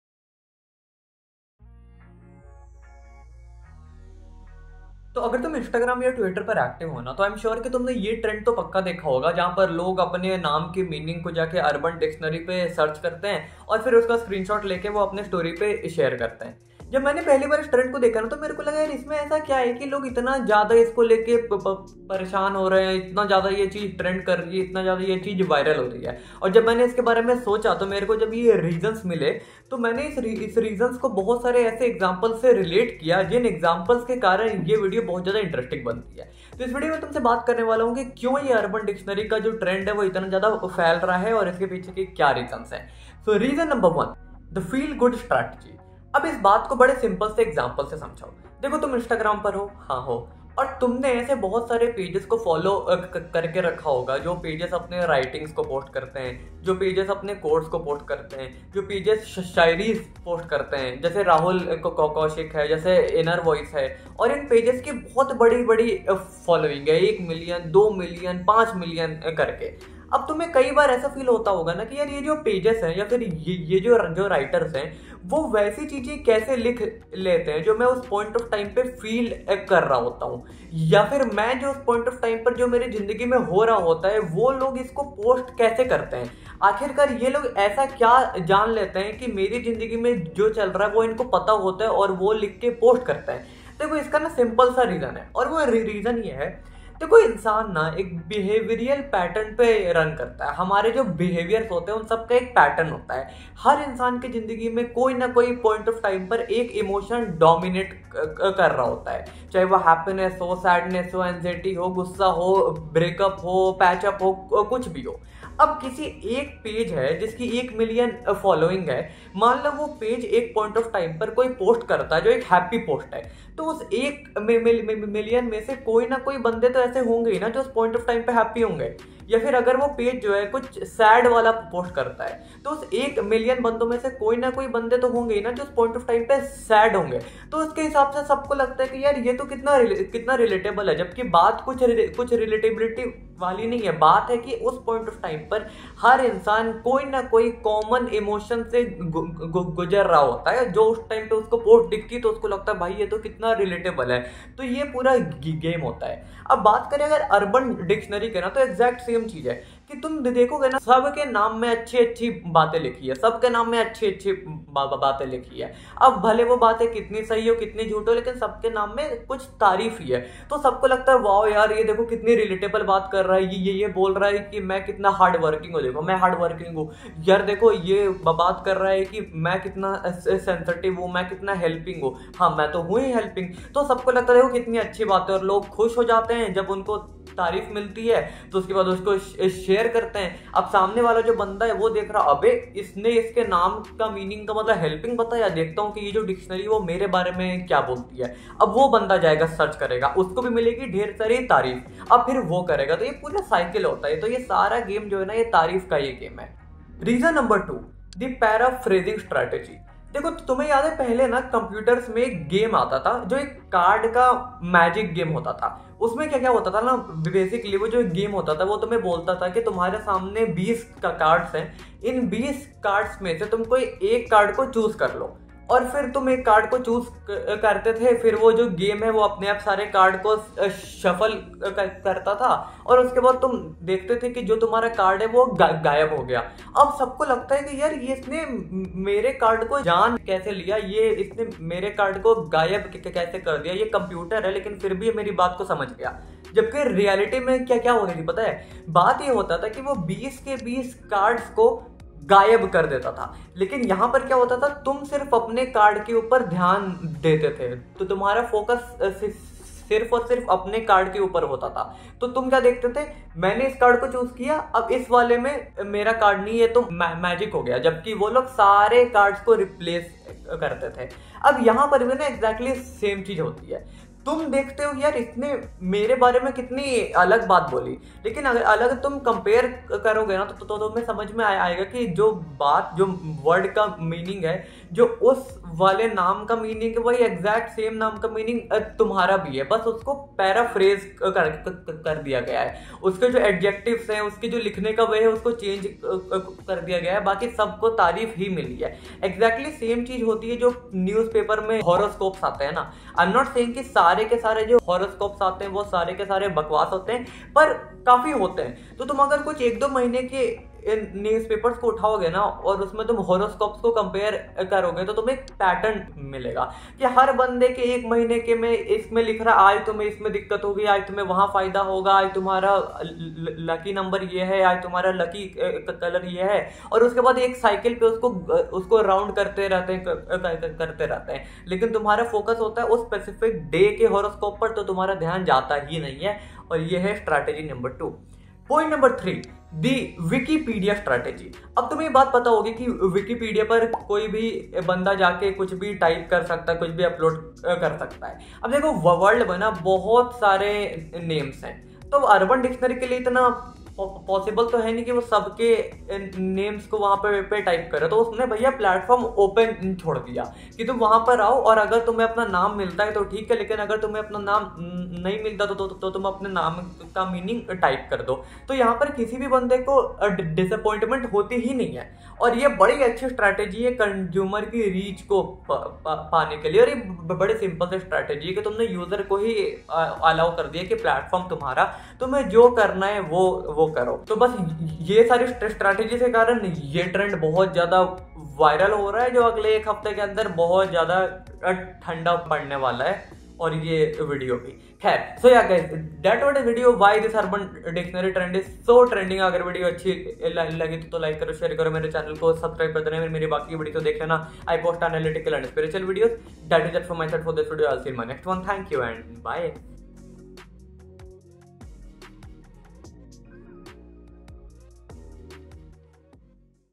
Why Urban Dictionary Name Meanings are getting VIRAL?
Urban Dictionary name meanings are getting viral on Instagram and twitter. Watch this video to know why!

Have a look at some of my best videos-